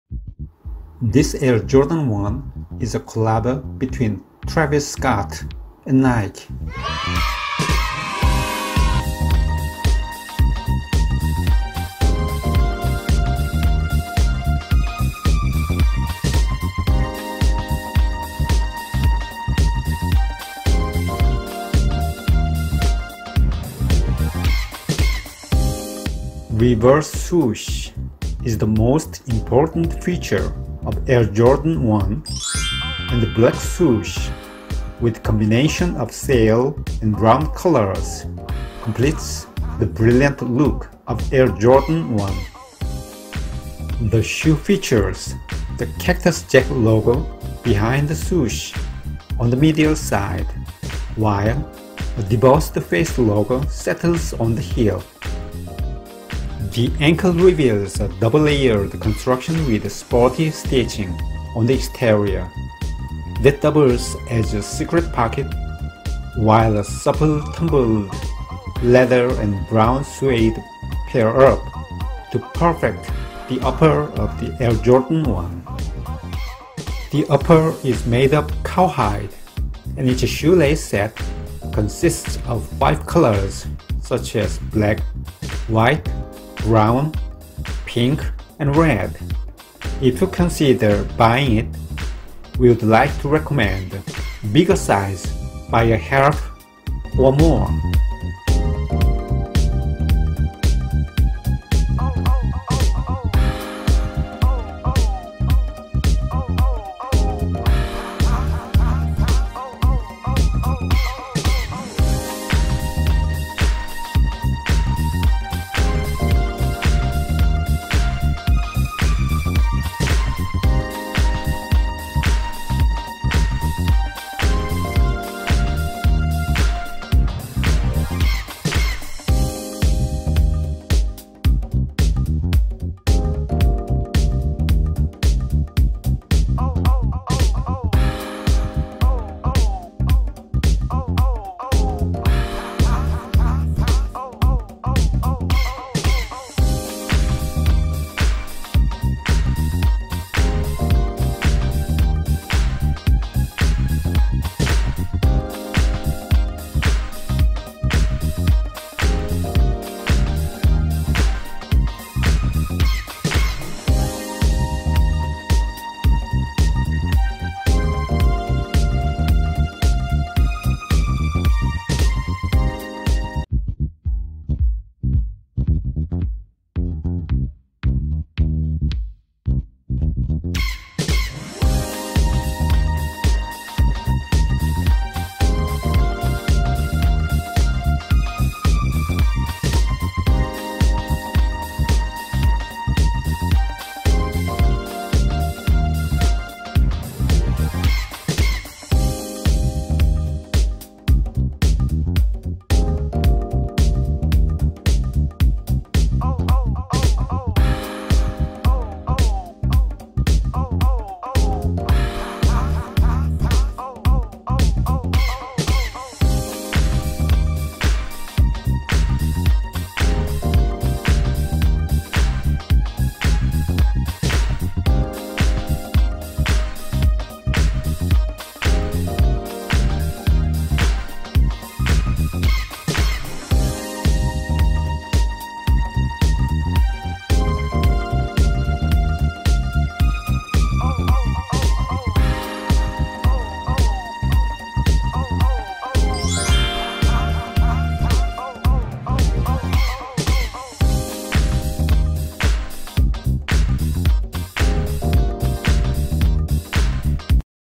Travis Scott X Nike Air Jordan 1 unboxing/Travis Scott Nike Air Jordan 1 on feet review
Style code CD4487-100
it's AUTHENTIC !